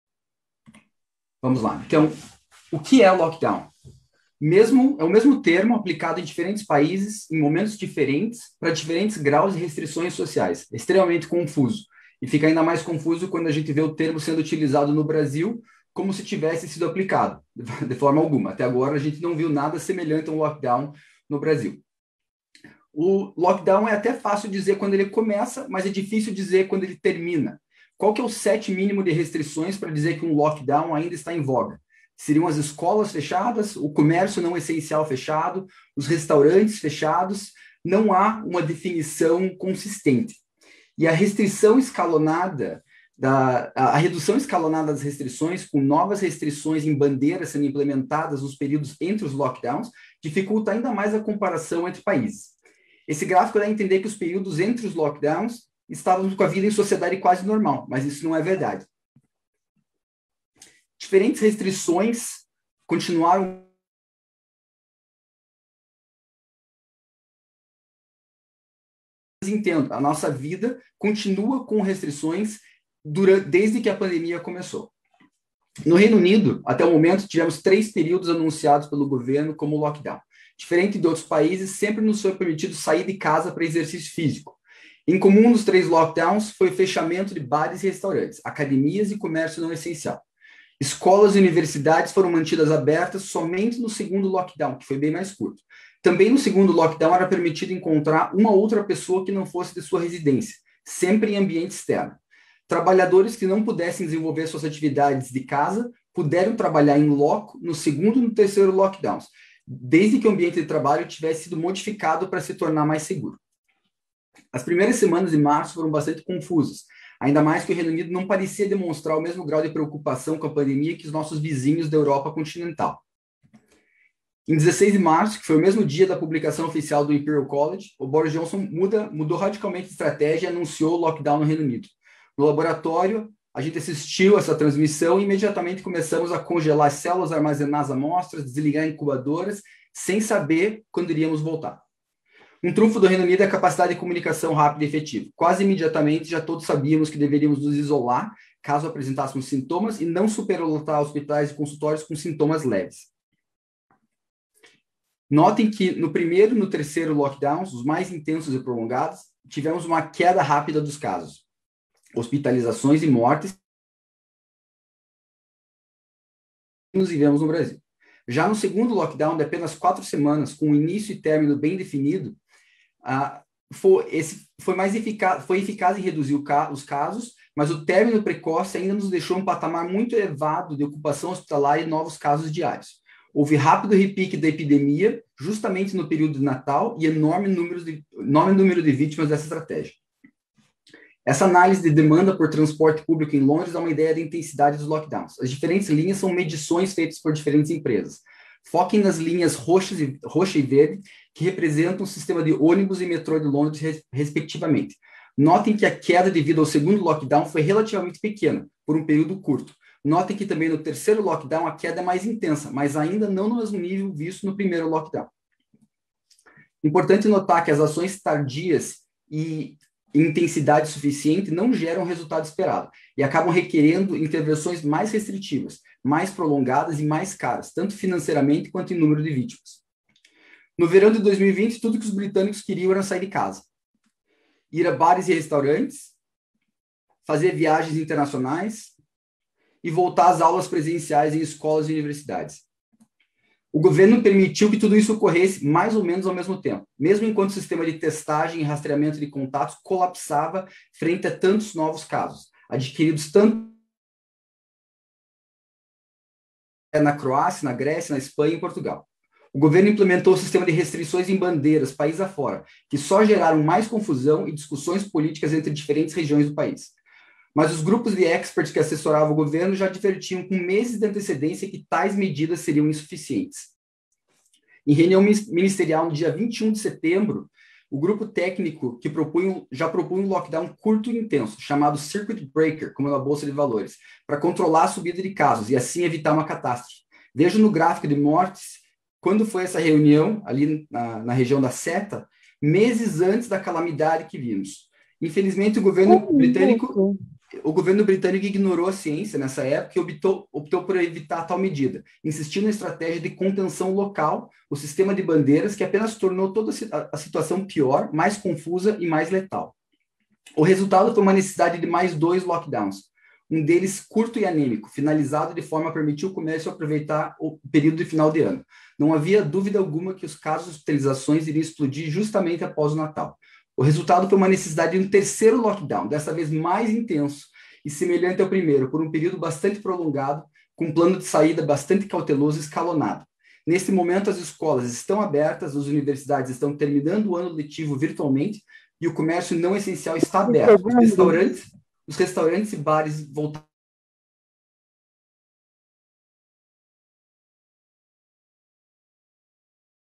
Pesquisador Ricardo Parolin mostra em gráficos a experiência do Reino Unido no combate à pandemia
Ricardo Parolin, médico e pesquisador da Universidade de Oxford, trouxe gráficos ao webinario sobre enfrentamento à pandemia, em 31/5, para apresentar a experiência internacional no combate à pandemia, com enfoque no Reino Unido.